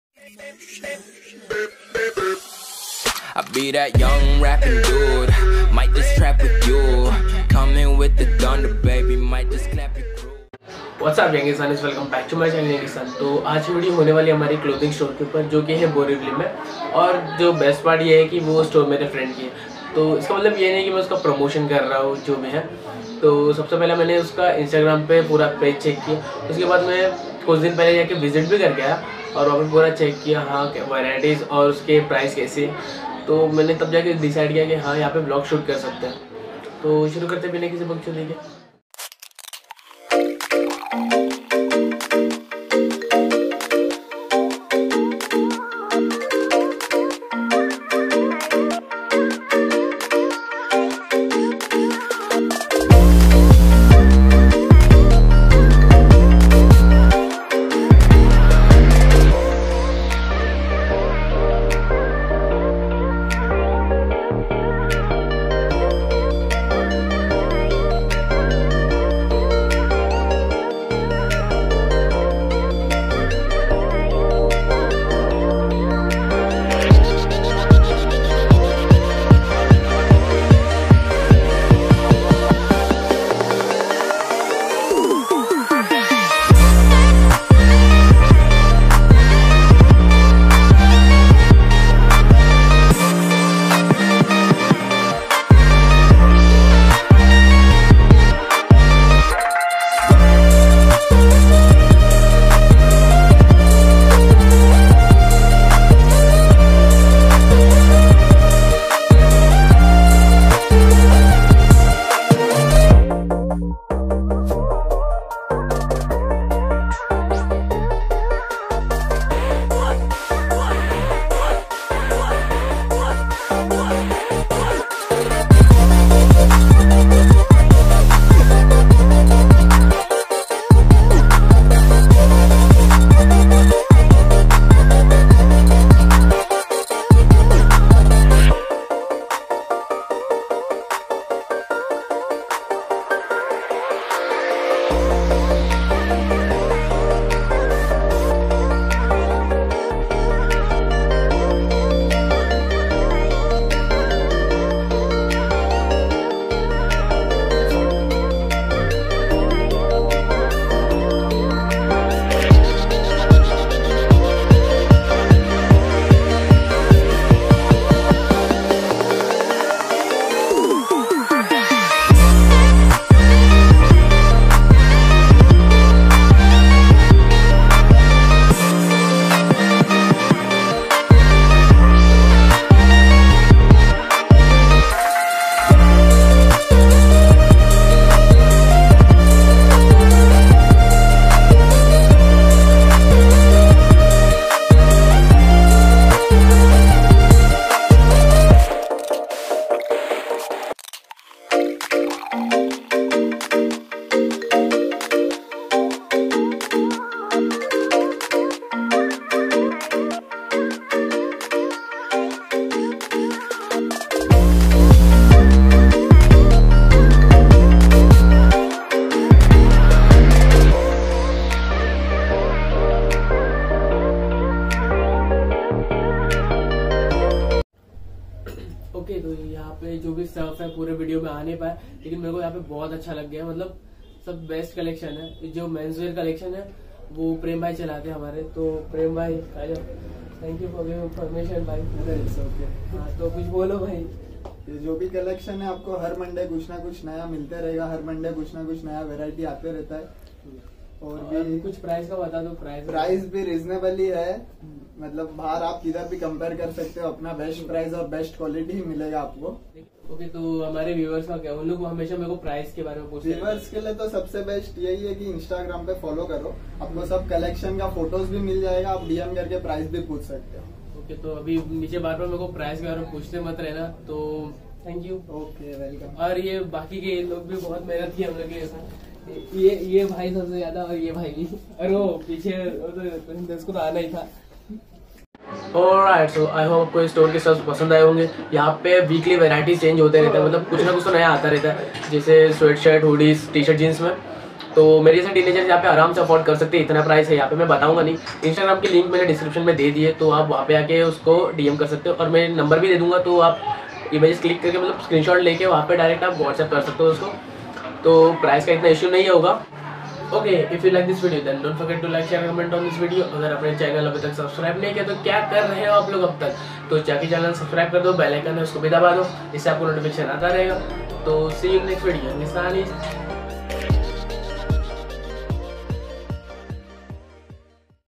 Evershine mens wear | Borivali east | Mumbai \Best price and quality
Hey wsuuuup youngistaanis

This video is all about fashion and clothing for men
So guys this was the best store i ever visit

Their collection is too good and and all are at such a reasonable price so you should visit here once

Address -  Near, Shop Number 15, Vitthal Nagar CHS, Western Express Hwy, Devipada, Borivali East, Mumbai, Maharashtra 400066

music credit -